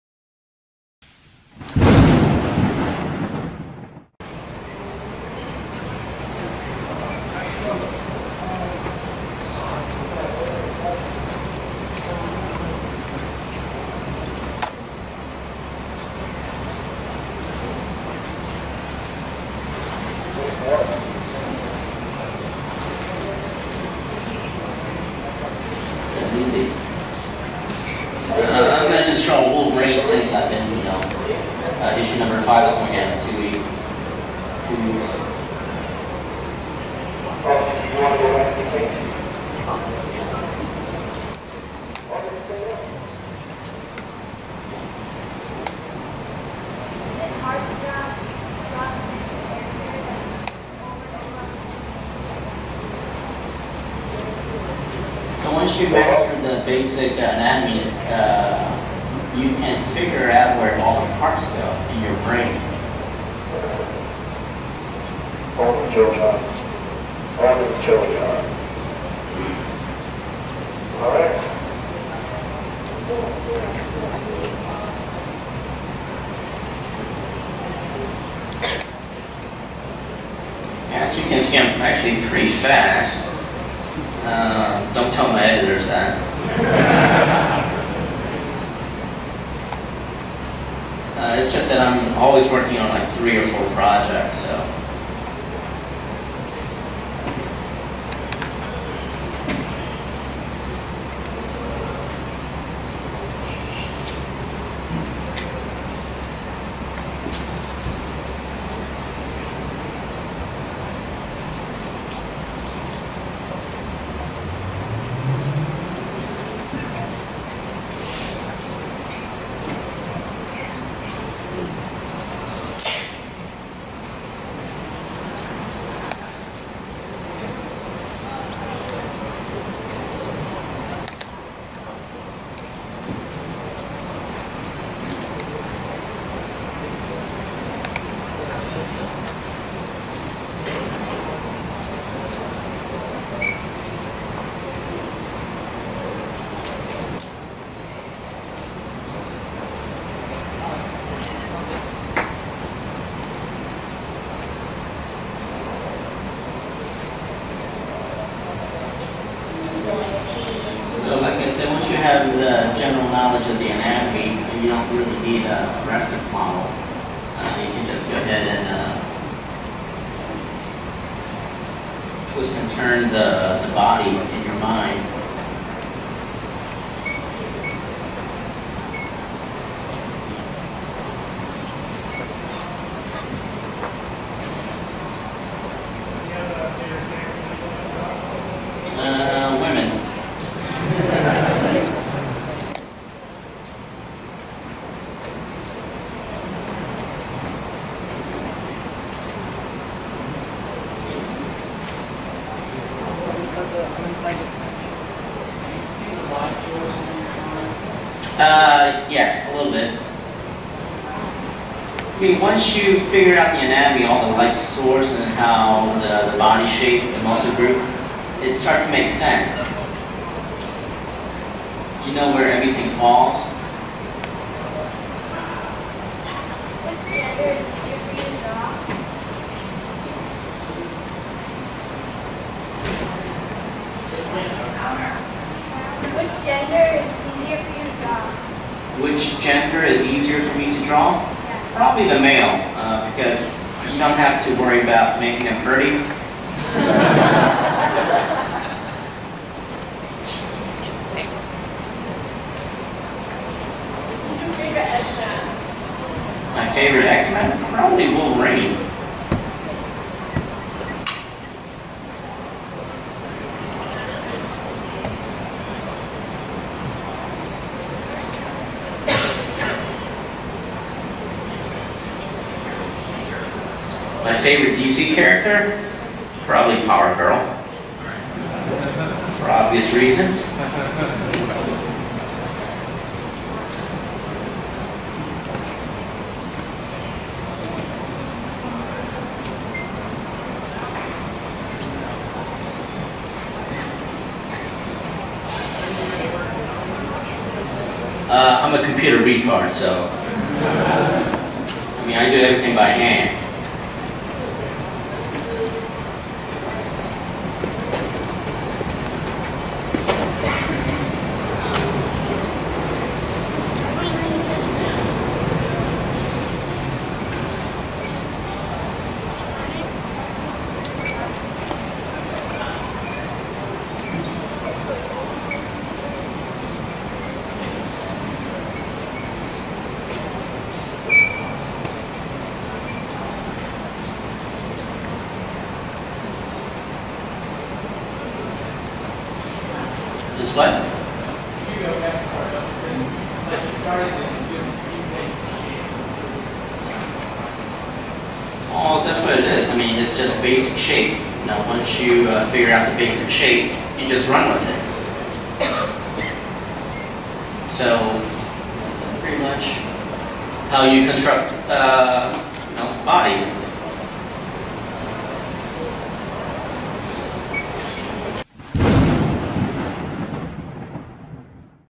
Frank Cho drawing Wolverine
Frank Cho drawing Wolverine at the 2013 Big Wow Comic-Fest!

Filmed and edited by Victor Glover